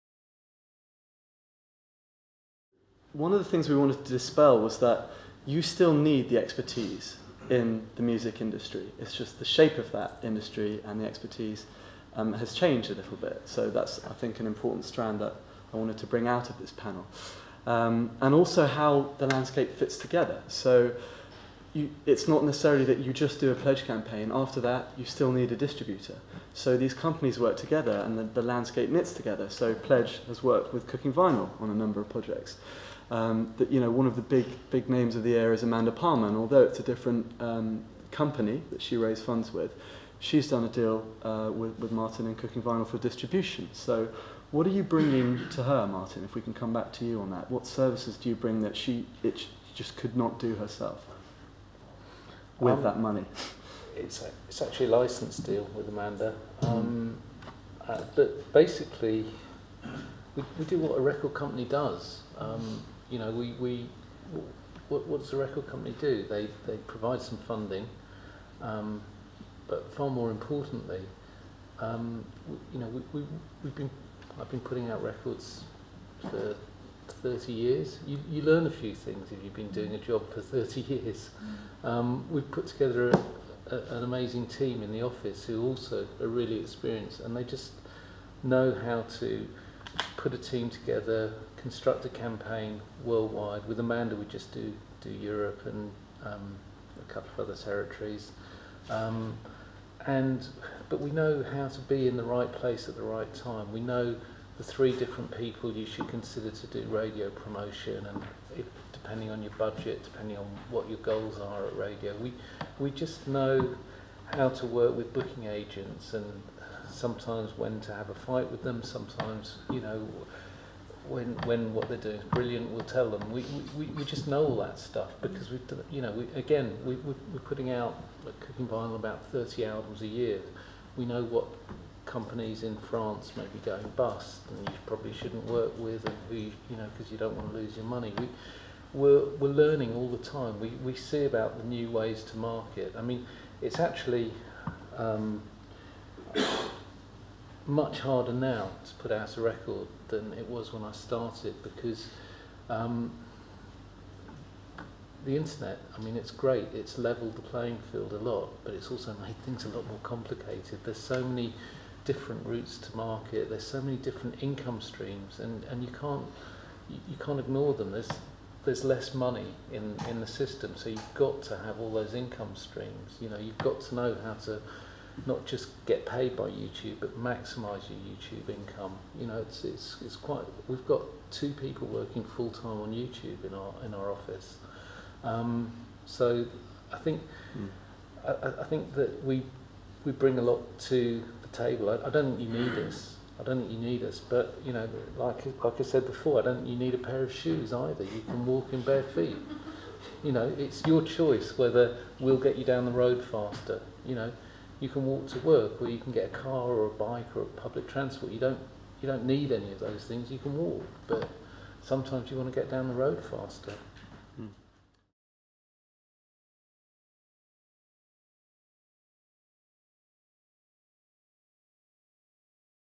Music Futures Conference - Future of Artist / Label Funding pt1
Music industry experts discuss the options for independent music businesses and artists to raise finance. Featuring Martin Goldschmidt (Cooking Vinyl), Alison Wenham (AIM), Paul Barton (Pledge Music) & Karim Fanous (Music Ally).

With such a strong history as one of the world's favourite independent record labels, Martin Goldschmidt explains why Cooking Vinyl and independent labels in general are best placed to run successful release campaigns and look after the best interests of artists.